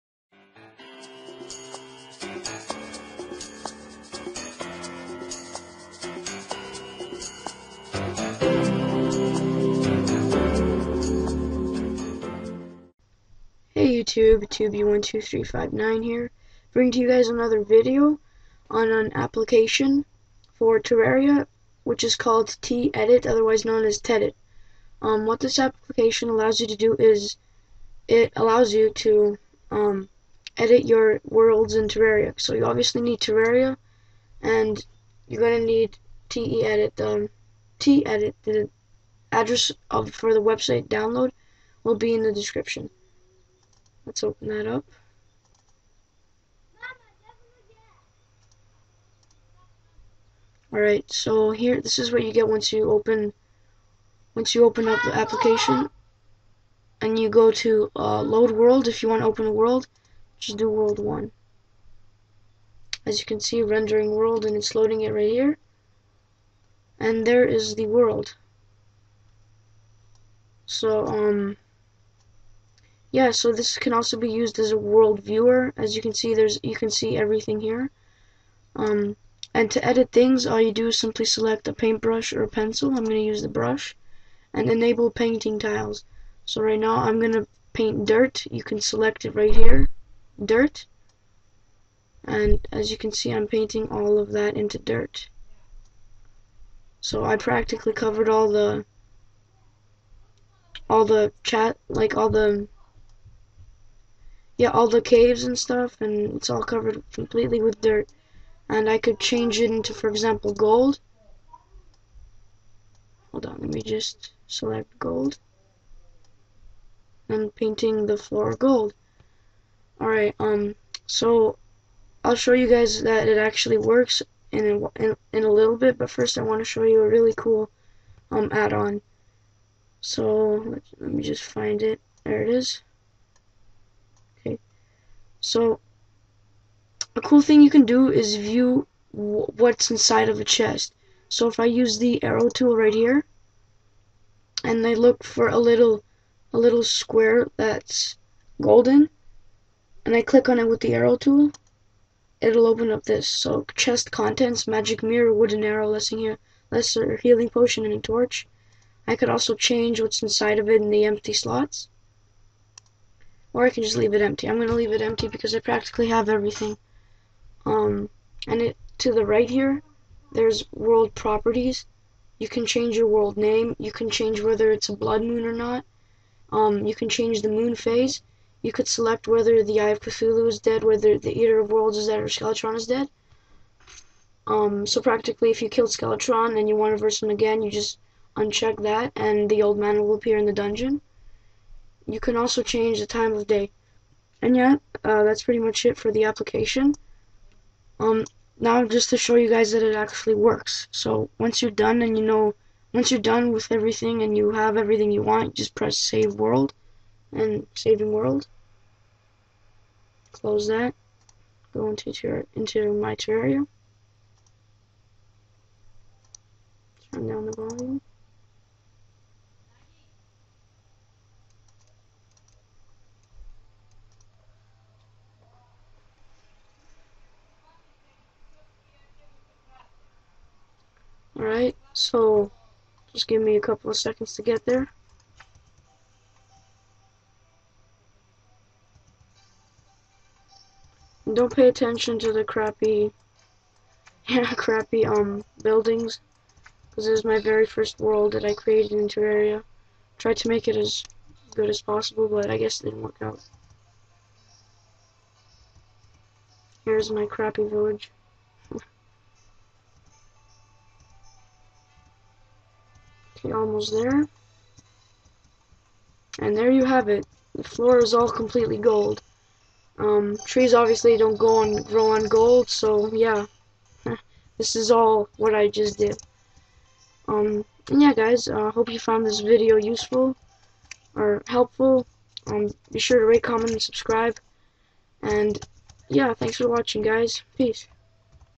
Terraria World Editor (TEdit)
Instuctions:
2nd: Download .Net Framework 4.0 If You Dont Have It.
3rd: Download The Editor.
4th: Got To Your Desktop And Find The Folder (TEdit).
5th: Open -TEdit-.